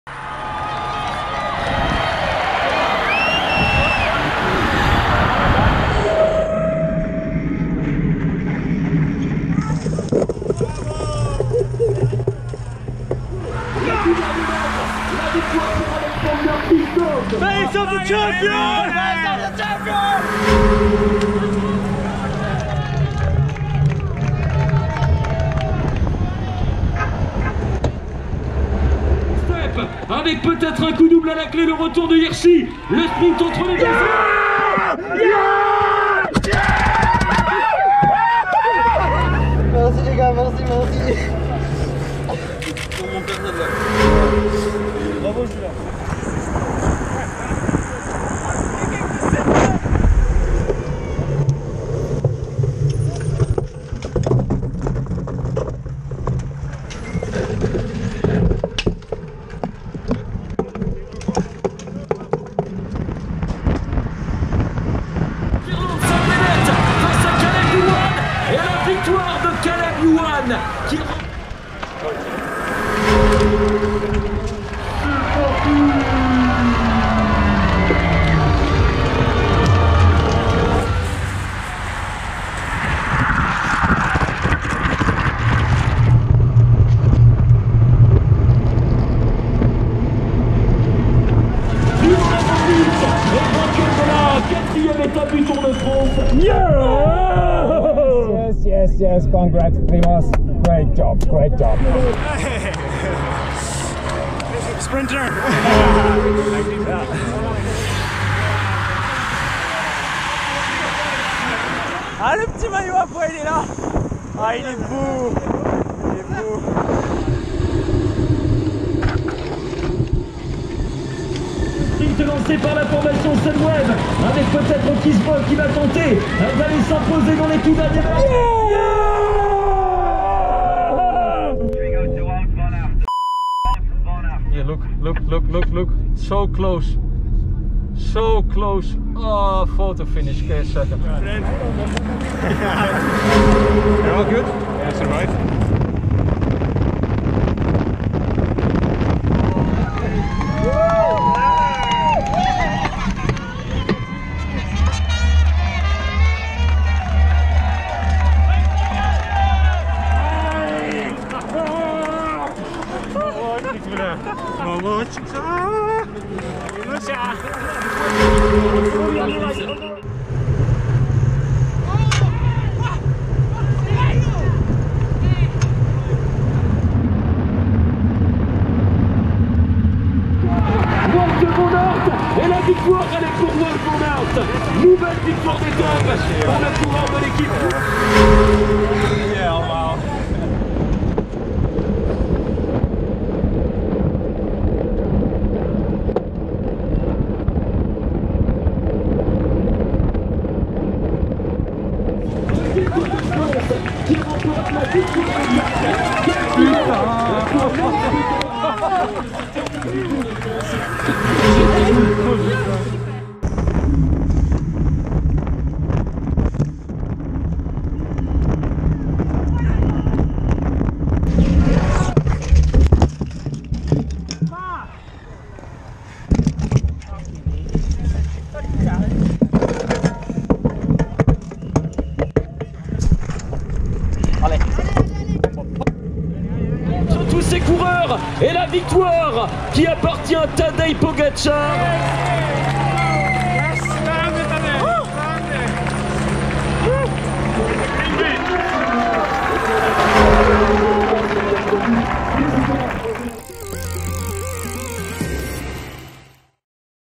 Tour de France 2020: The best on-bike action from Stages 1-9
Relive Stages 1-9 of the Tour de France 2020 with the very best of our on-bike action!